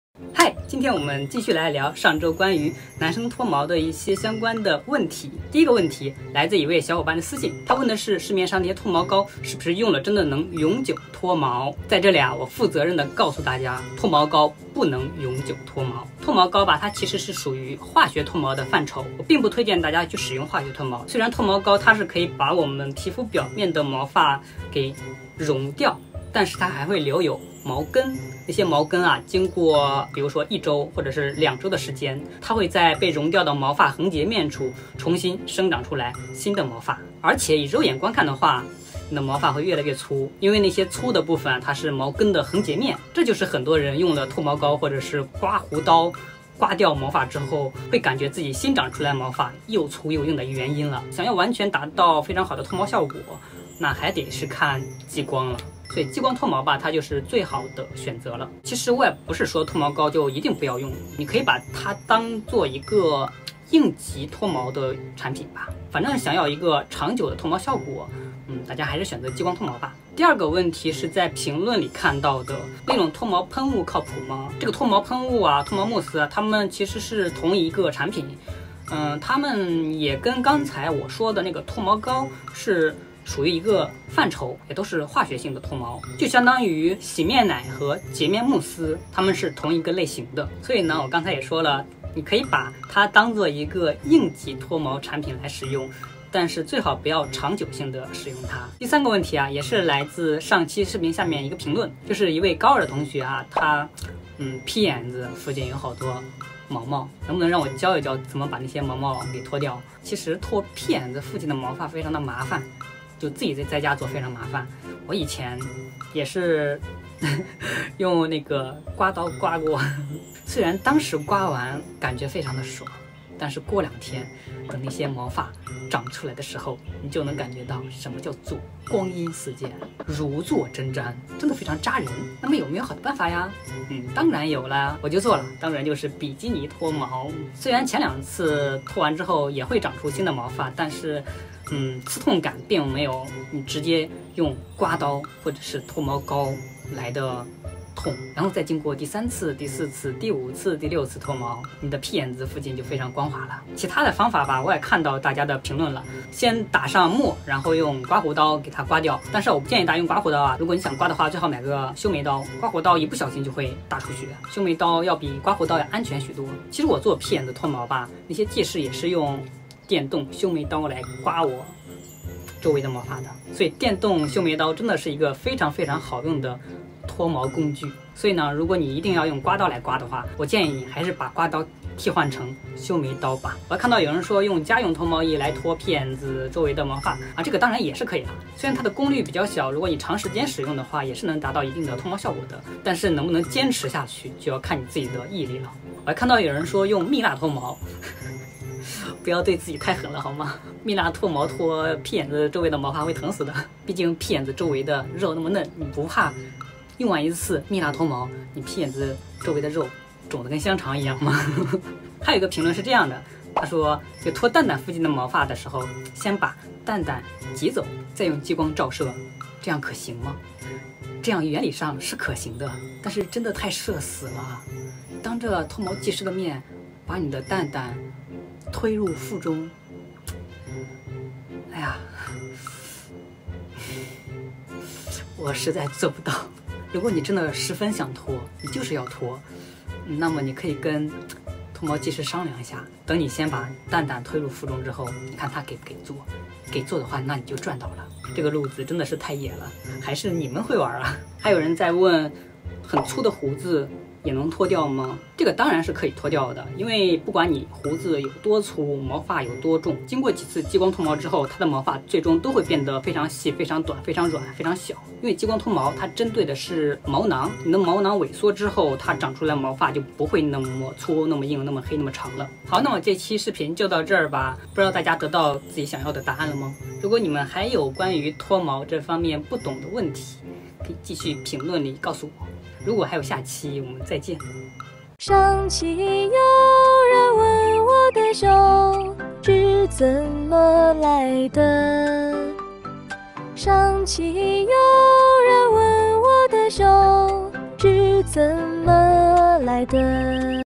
【木木】男生脱毛问答（二）！屁眼子附近的毛怎么脱？
别问了，是臭男人！
喜欢女生，喜欢女装，没有女朋友！

这里是木木，我会给大家同步更新各种妆容和女装视频
请大家多多支持！